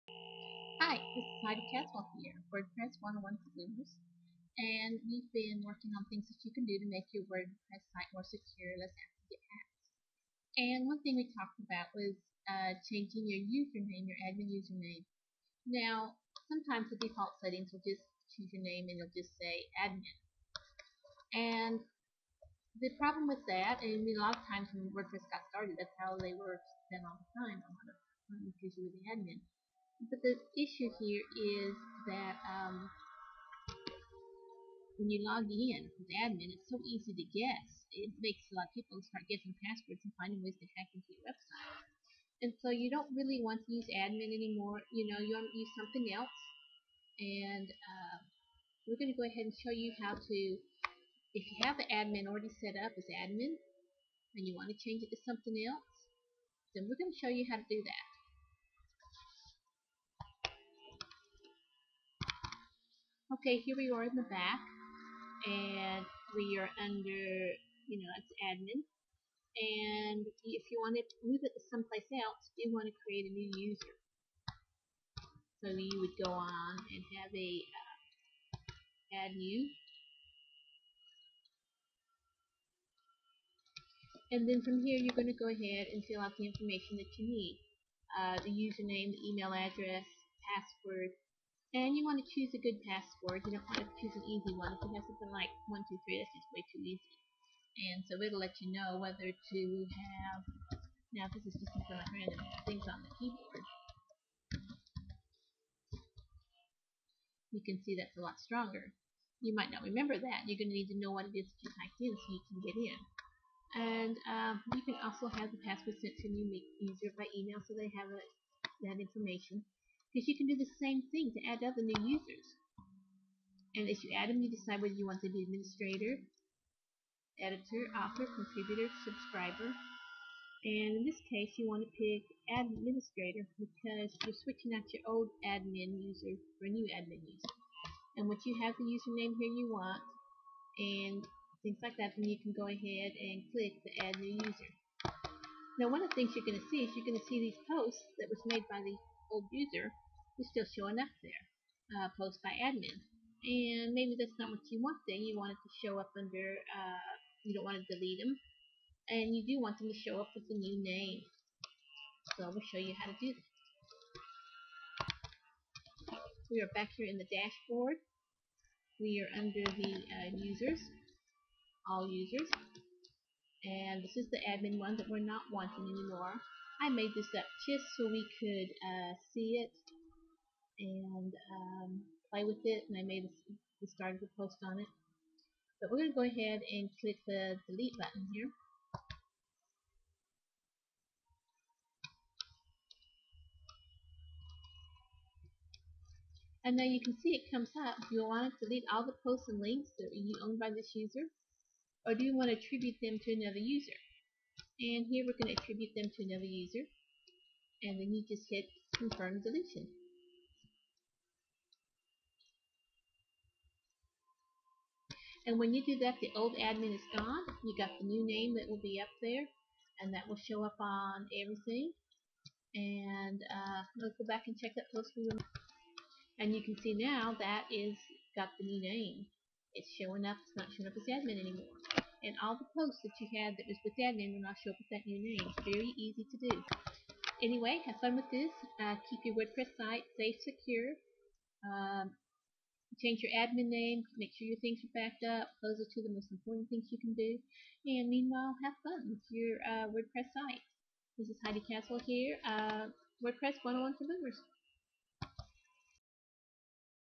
Changing Admin User Name in Wordpress
How to change the admin username in wordpress from "admin" to make your site more secure.   More tutorials on securing your wordpress site are